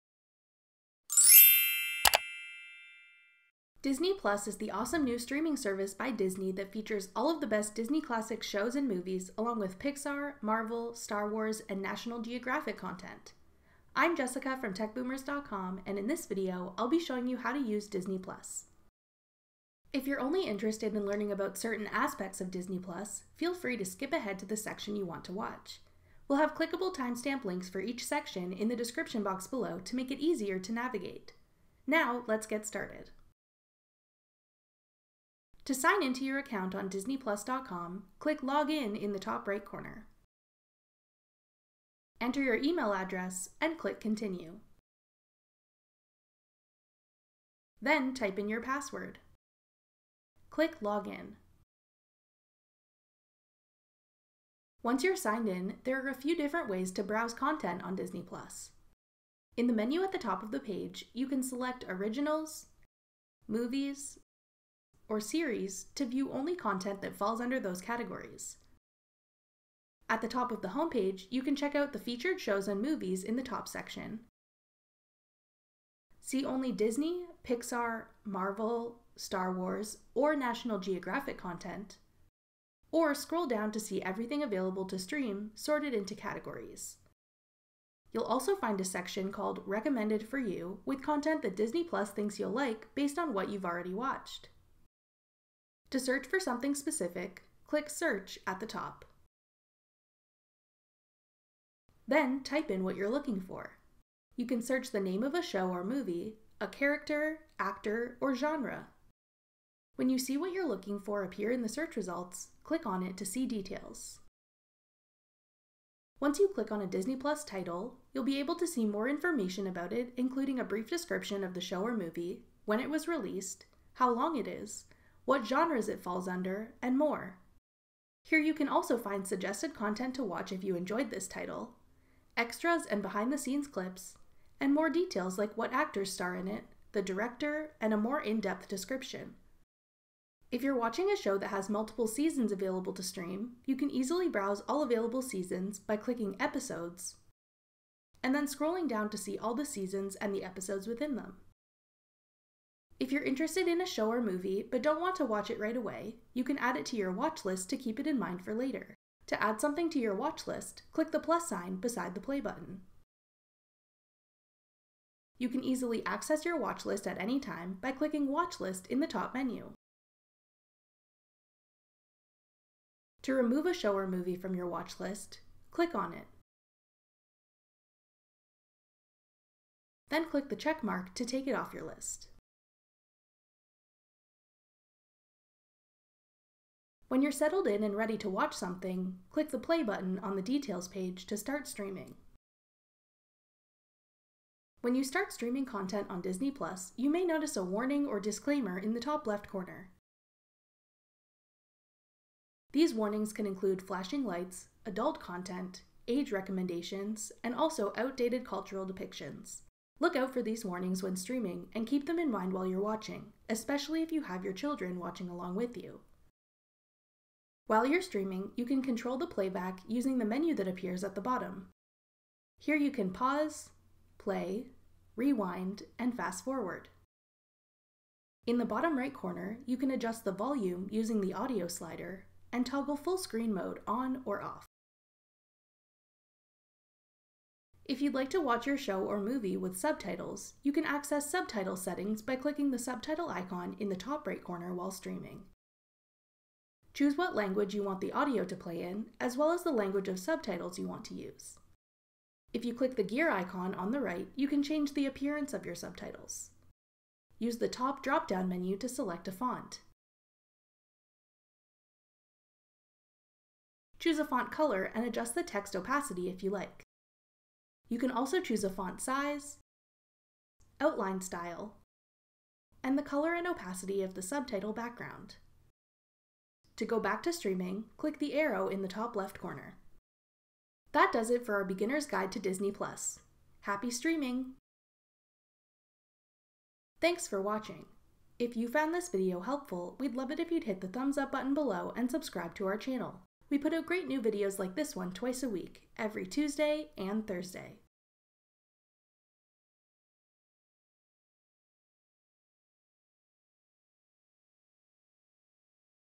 How to Use Disney+ | Beginner's Guide
Disney+ is the awesome new streaming service by Disney that features all of the best Disney classic shows and movies, along with Pixar, Marvel, Star Wars, and National Geographic content. Watch this video to learn how to use it!

Topic Timestamps:
Find Content to Watch - 0:54
Watchlist - 2:32
Content Playback/Controls - 3:06
Subtitles - 3:57

To sign into your account on DisneyPlus.com, click Log In in the top right corner. Then, type in your password. Click Log In.

There are a few different ways to browse content on Disney+. In the menu at the top of the page, you can select Originals, Movies, or Series to view only content that falls under those categories. You can check out the featured shows and movies in the top section, see only Disney, Pixar, Marvel, Star Wars, or National Geographic content, or scroll down to see everything available to stream, sorted into categories. You’ll also find a section called “Recommended For You” with content that Disney+ thinks you’ll like based on what you’ve already watched.

To search for something specific, click Search at the top. Then, type in what you’re looking for. You can search the name of a show or movie, a character, actor, or genre. When you see what you’re looking for appear in the search results, click on it to see details.

If you’re watching a show that has multiple seasons available to stream, you can easily browse all available seasons by clicking “Episodes” and then scrolling down to see all the seasons and the episodes within them.

If you’re interested in a show or movie but don’t want to watch it right away, you can add it to your Watchlist to keep it in mind for later. To add something to your watch list, click the plus sign beside the play button. You can easily access your watchlist at any time by clicking Watchlist in the top menu. To remove a show or movie from your Watchlist, click on it, then click the check mark to take it off your list.

When you’re settled in and ready to watch something, click the Play button on the details page to start streaming.

When you start streaming content on Disney+, you may notice a warning or disclaimer in the top left corner. These warnings can include flashing lights, adult content, age recommendations, and also outdated cultural depictions. Look out for these warnings when streaming and keep them in mind while you’re watching, especially if you have your children watching along with you.

While you’re streaming, you can control the playback using the menu that appears at the bottom. Here you can pause, play, rewind, and fast forward. In the right corner, you can adjust the volume using the audio slider, and toggle full screen mode on or off.

If you’d like to watch your show or movie with subtitles, you can access subtitle settings by clicking the subtitle icon in the top right corner while streaming. Choose what language you want the audio to play in, as well as what language of subtitles you want to use.

If you click the gear icon on the right, you can change the appearance of your subtitles. Use the top drop-down menu to select a font. Select a font colour and adjust the text opacity if you like. You can also choose a font size, outline style, and the colour and opacity of the subtitle background.